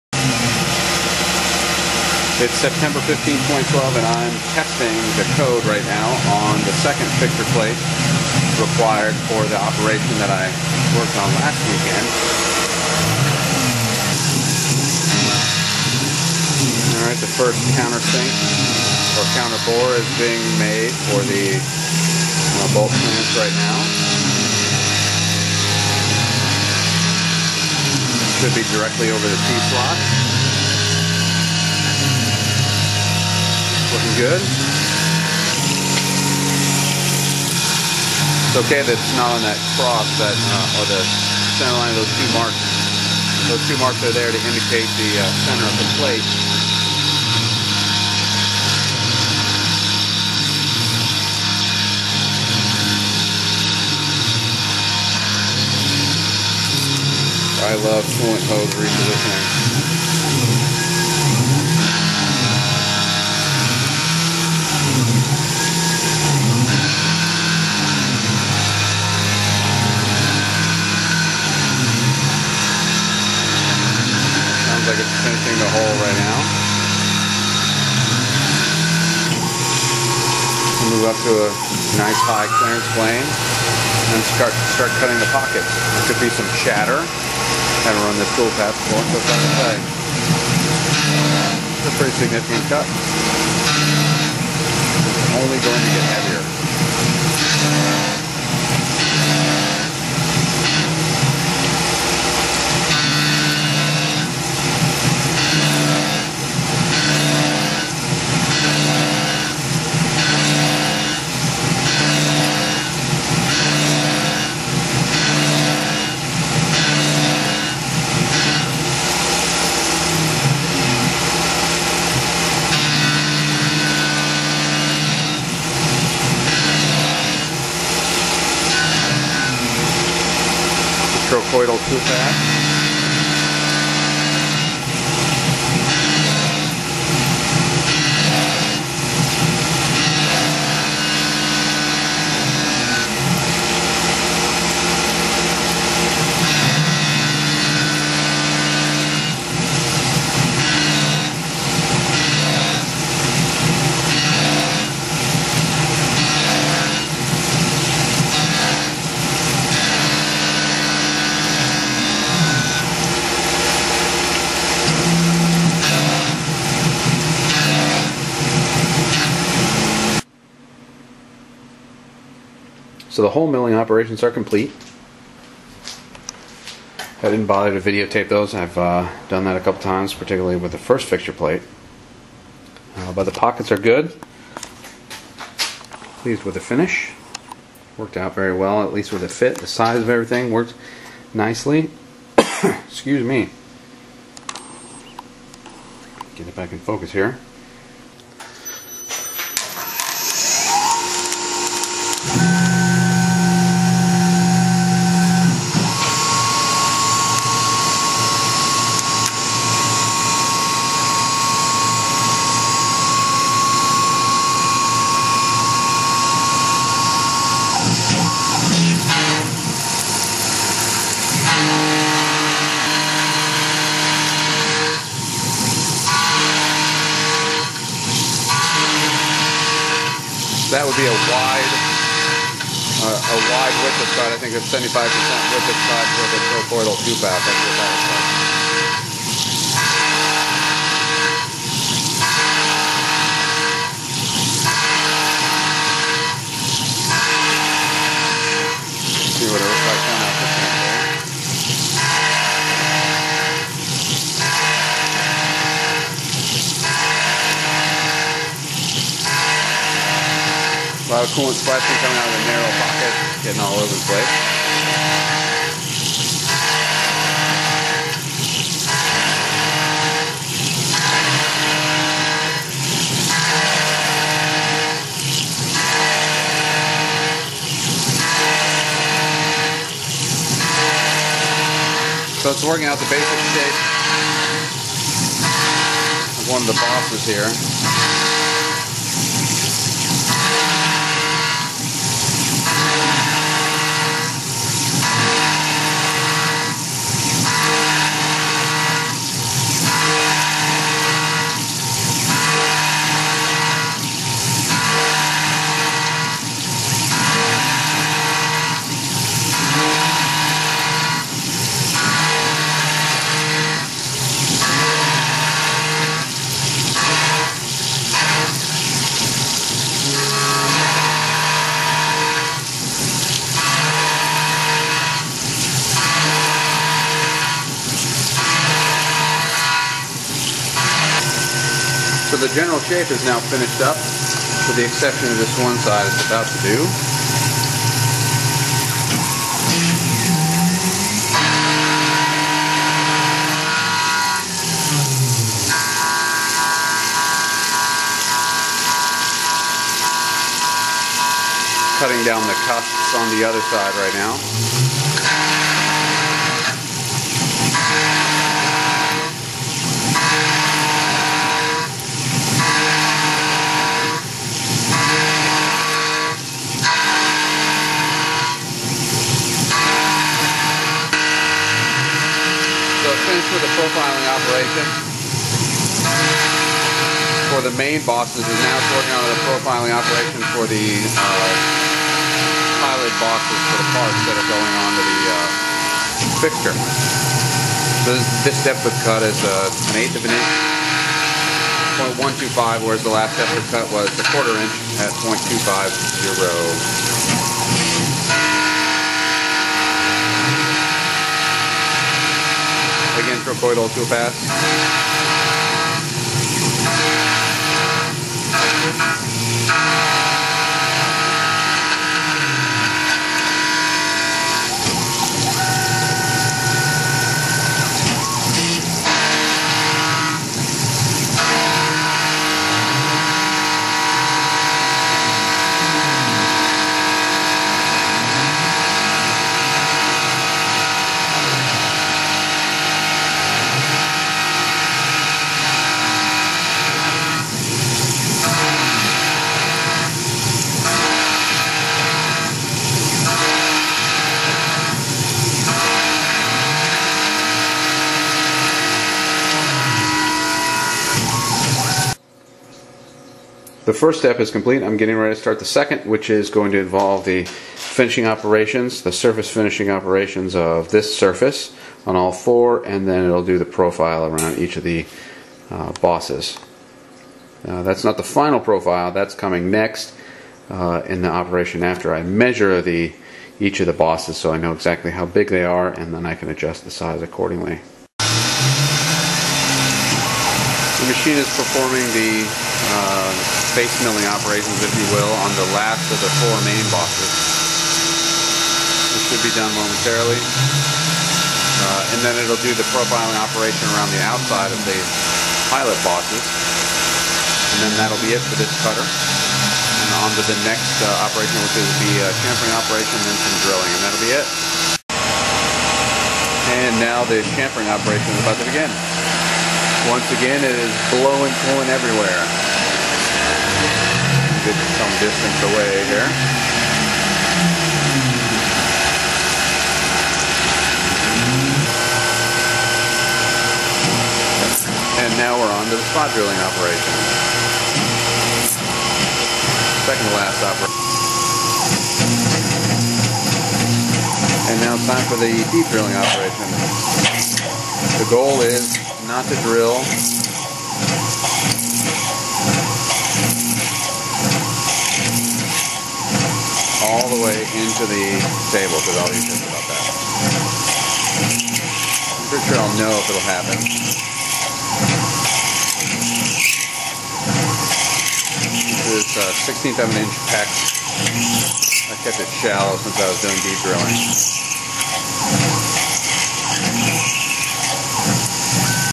Tormach PCNC 1100 - Fixture Plate CNC Milling
Please pardon the rough editing.  I'd prefer to be in the shop making parts than spending four hours making the video pretty.

This video shows the CNC machining of a second fixture plate to make bushing housings.  The fixture is designed to hold 8 parts simultaneously, just like the first fixture.

Five tools were used; 1/2" 3 flute rougher, 3/8" 2 flute end mill (profiling and "face milling"), 1/4" spot drill, 5/16" drill (hole prep for hand tapping).  Trochoidal tool paths were used on full width roughing cuts.